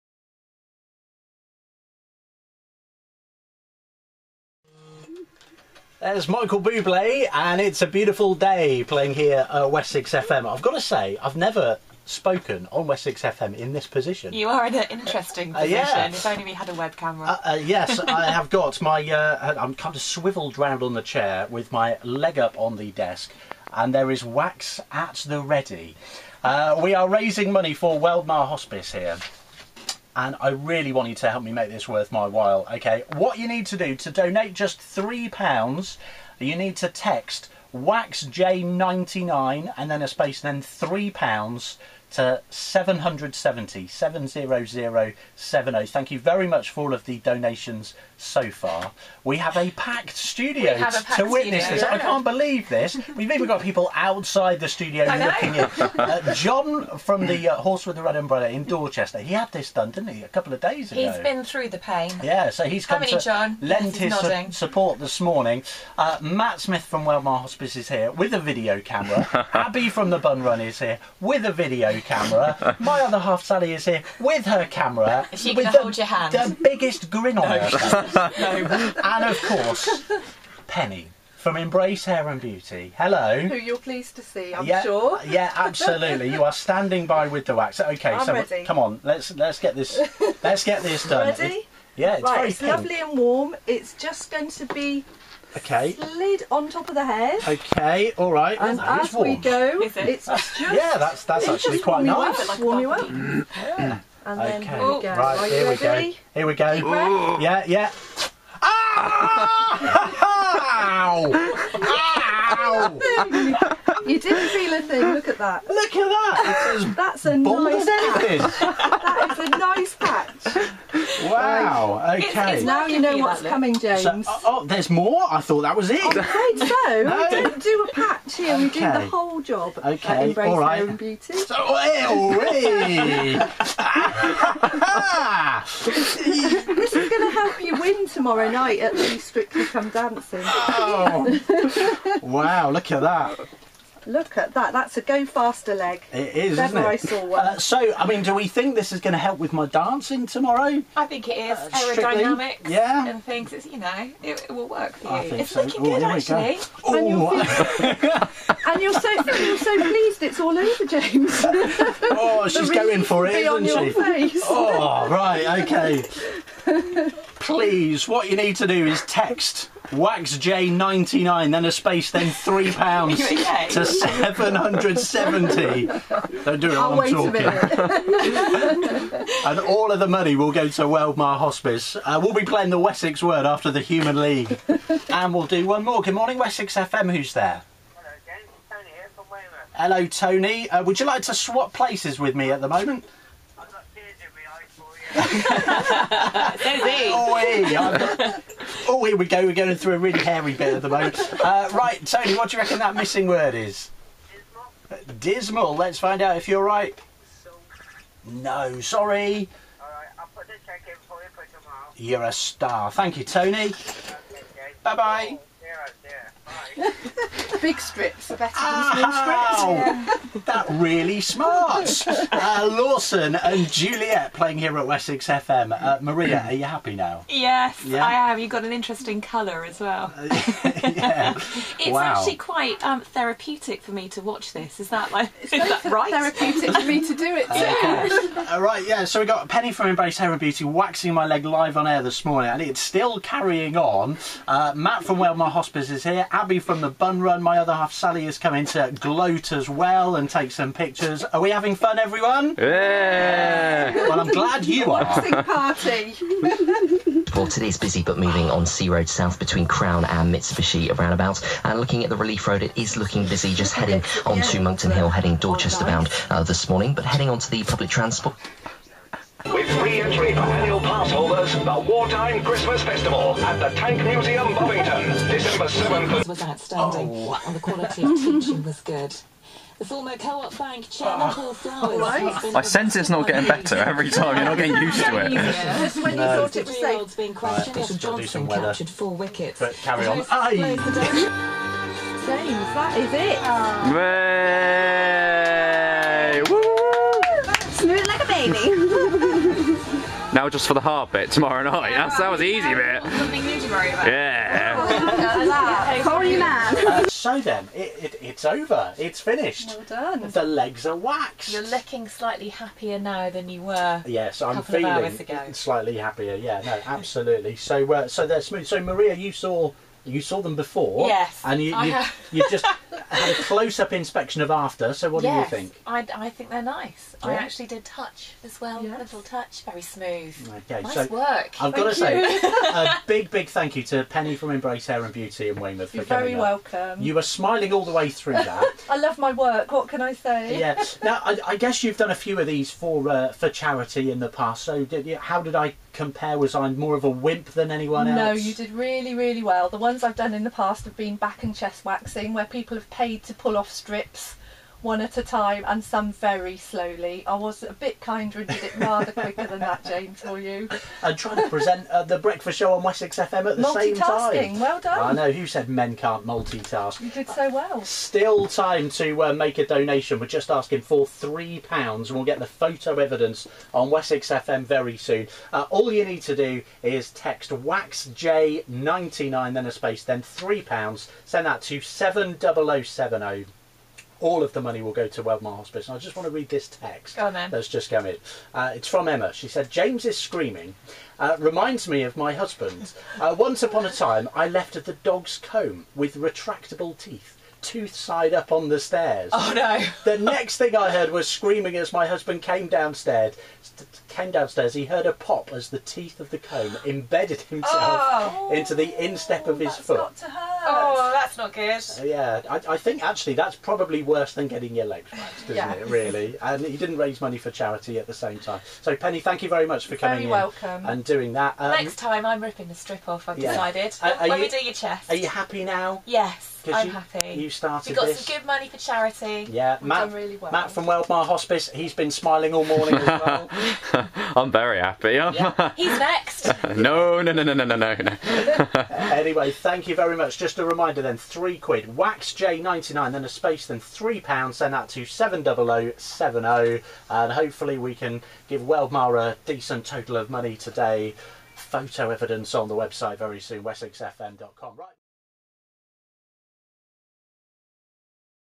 Leg waxing for Weldmar
On 22 November 2013, James O'Neill of WessexFM had his legs waxed in aid of Weldmar Hospicecare Trust as part of his fundraising for the Strictly Ballroom competition being held by Weldmar on Saturday 23rd November.